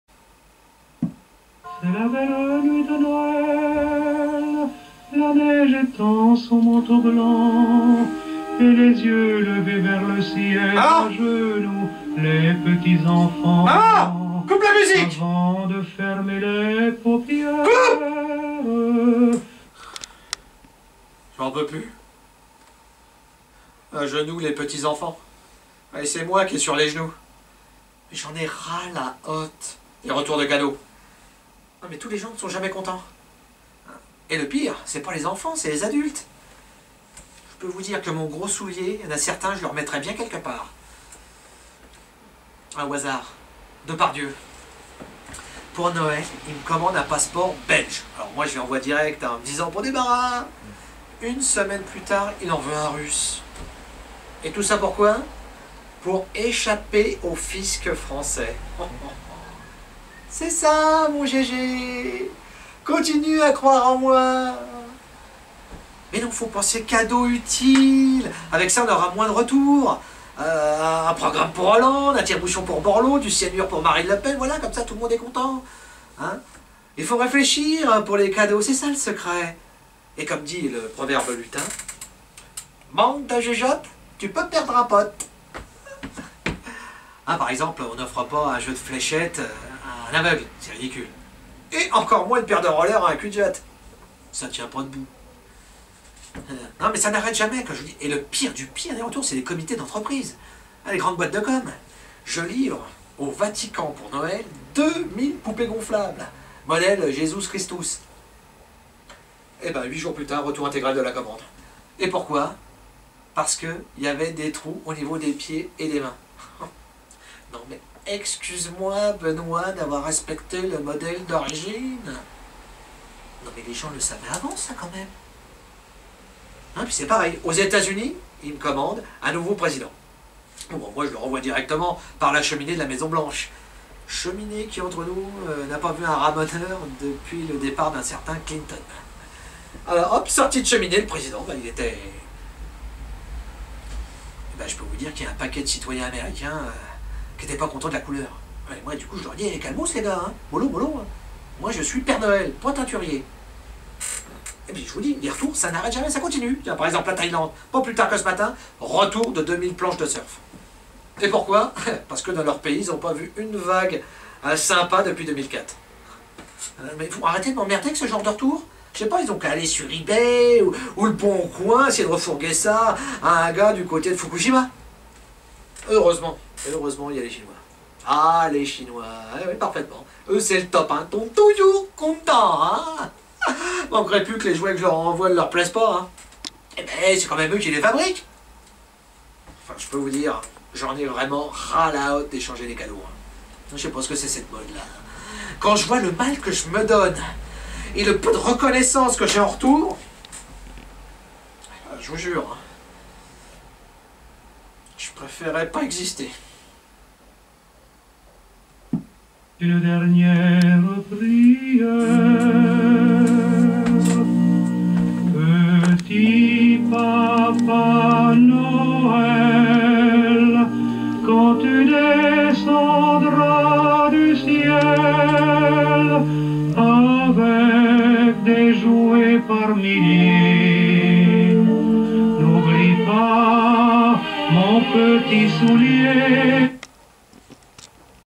J'ai échangé mes cadeaux de Noël
Sketch de sélection pour l'émission ONDAR qui à pour thème "J'ai échangé mes cadeaux de Noël"... Ecriture Jeff Didelot et Laurent Mentec